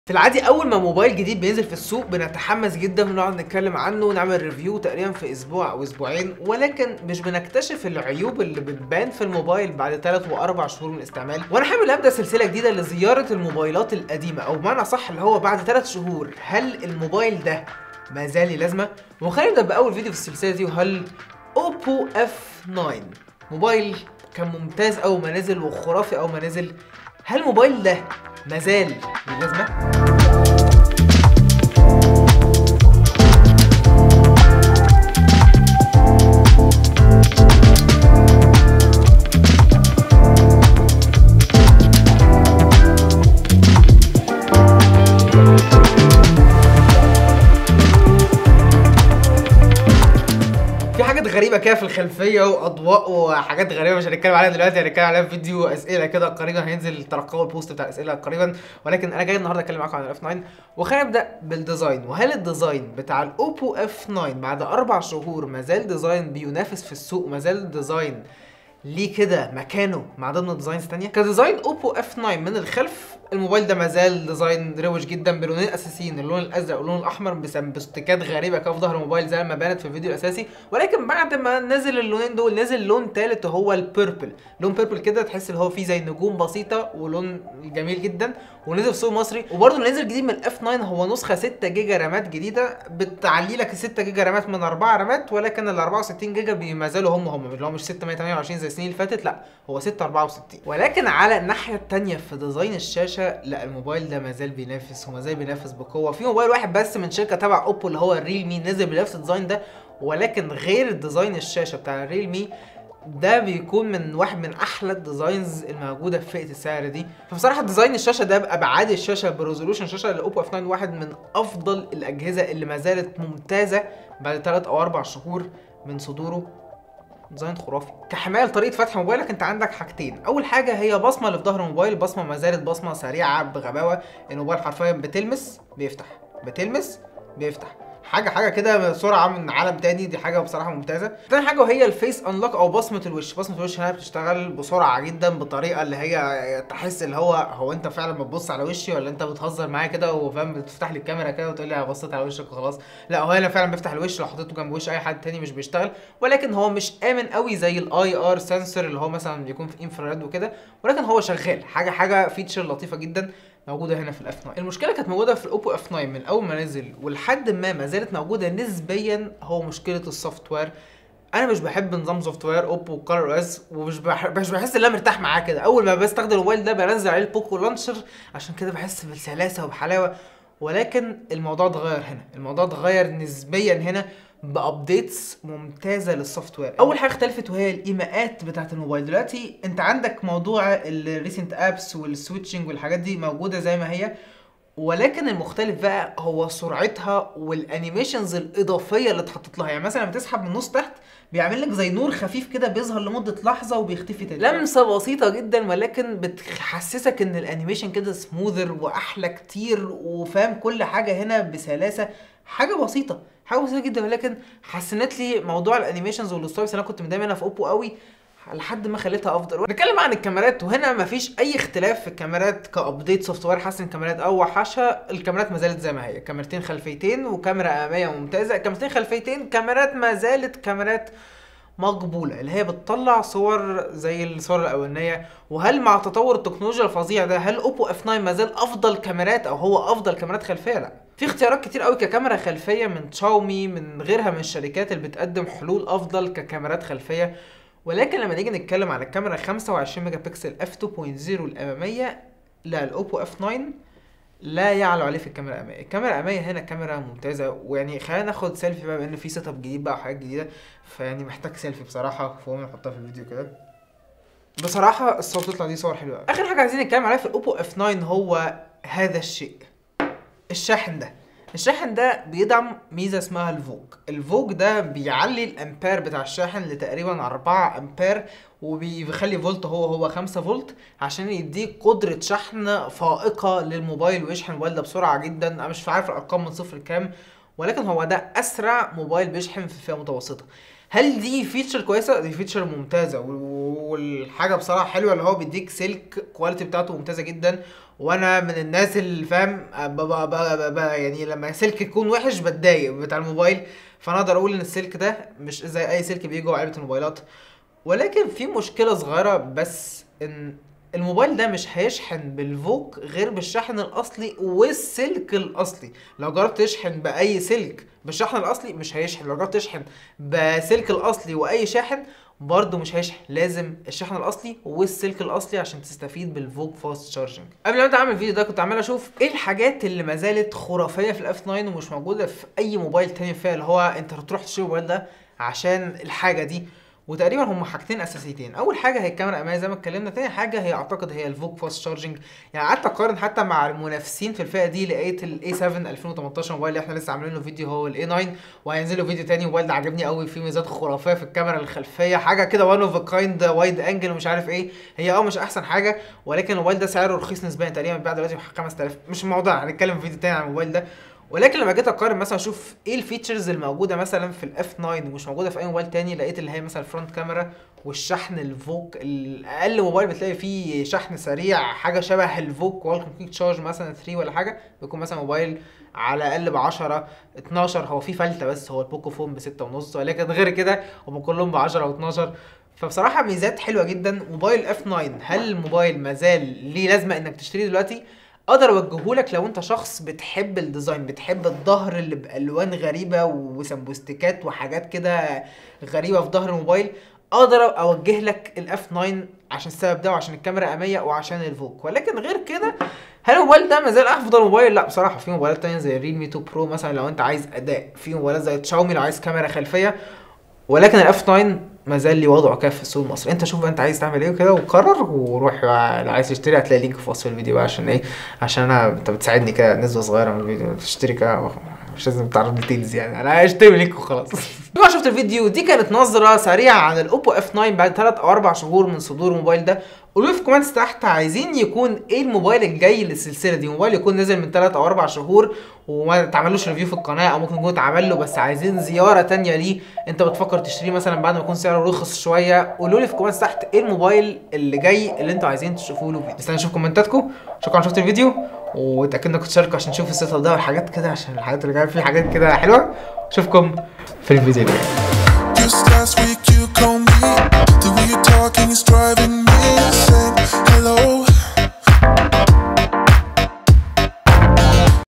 هل Oppo F9 مازال_له_لازمه - بعد ثلاثة شهور استخدام ؟
اوبو اف 9 مازال له لازمه ؟

شارك الفيديو عشان انت جميل :)

عني:
القناه ديه بالنسبالي هي المكان الي بحب ارغي فيه عن الحاجات الي بحبها.
عادة بتكون تكنولوجيا بس ساعات كتير بتكون Vlogs وانواع مختلفة من المحتوى.